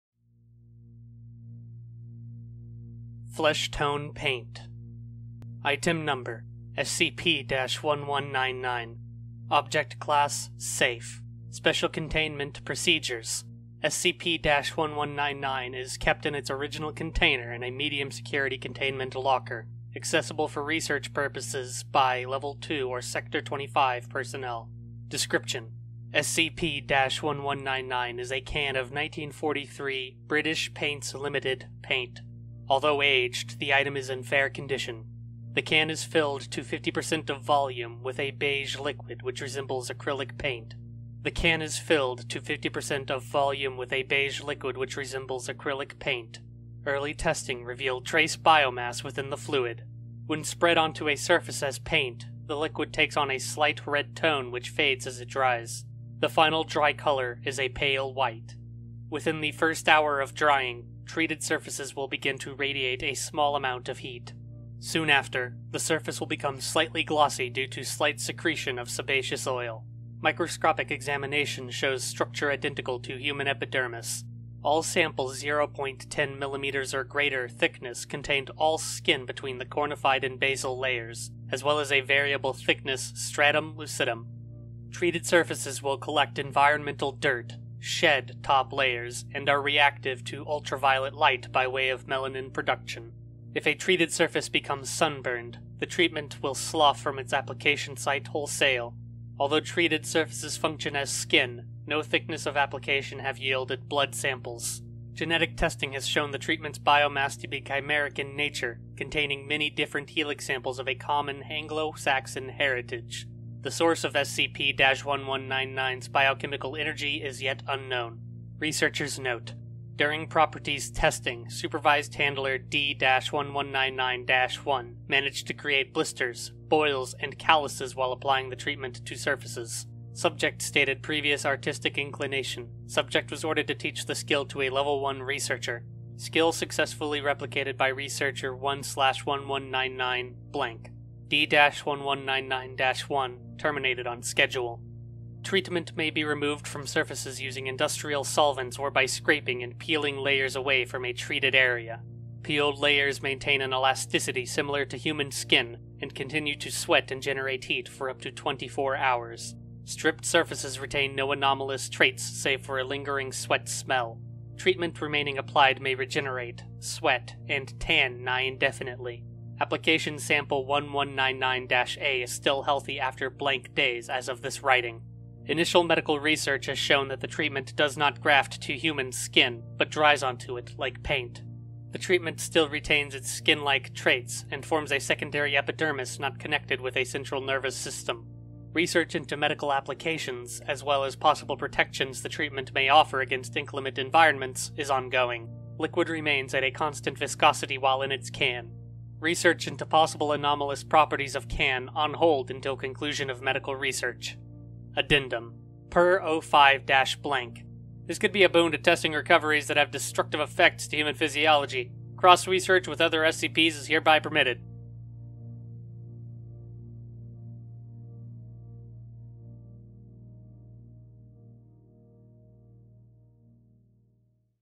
SCP-1199 Fleshtone Paint [Safe]
Taking finger painting a step too far.

SCP by "Psyshade".